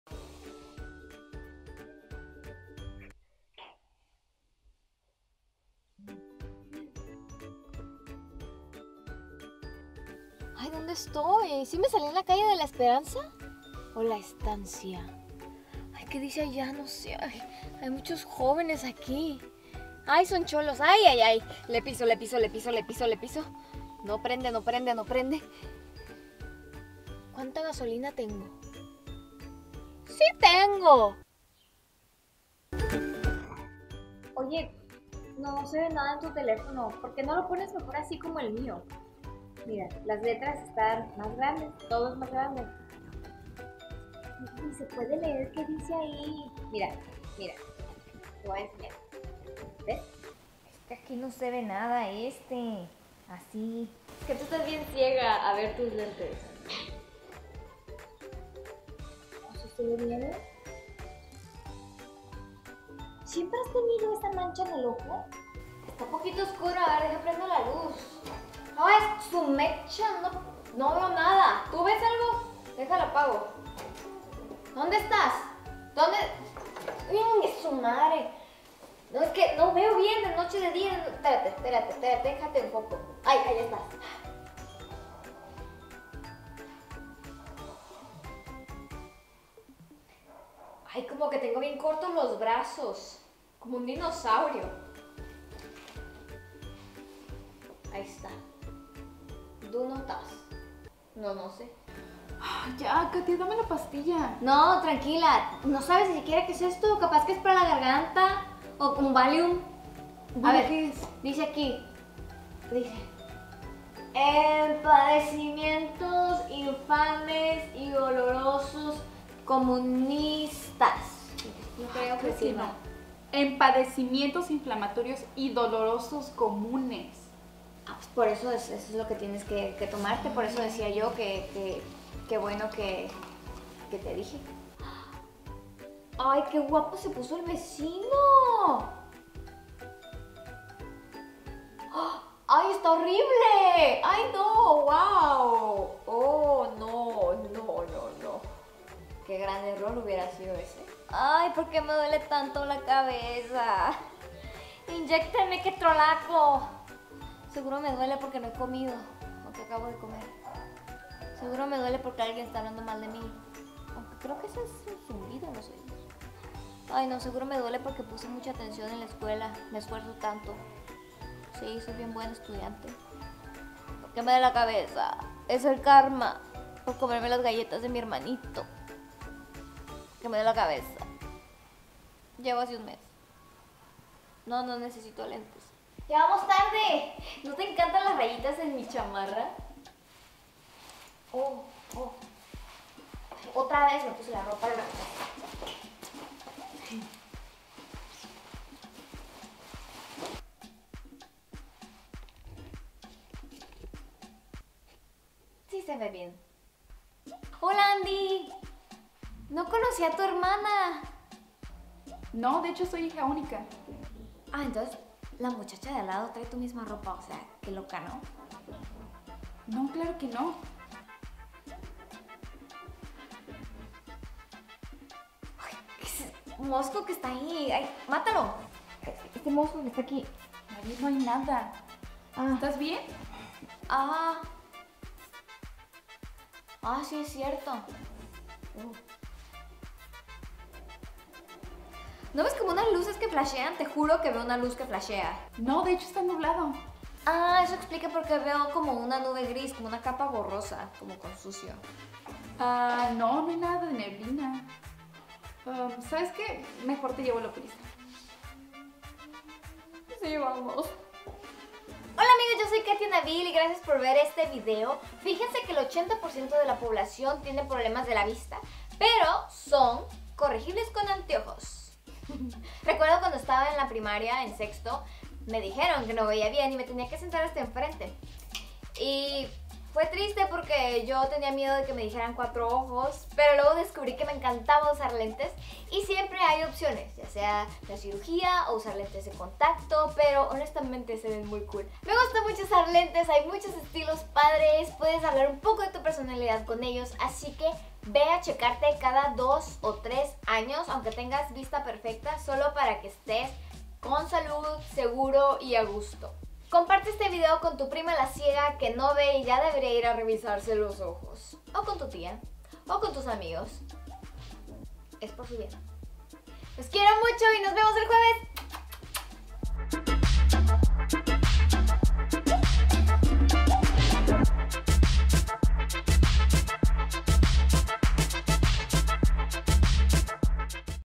SEÑALES DE QUE NECESITAS LENTES / GAFAS - Katia Nabil
Miopía, astigmatismo, vista cansada… Si llevas dias con ardor de ojos, vista borrosa... Aquí tienes las señales de que necesitas gafas.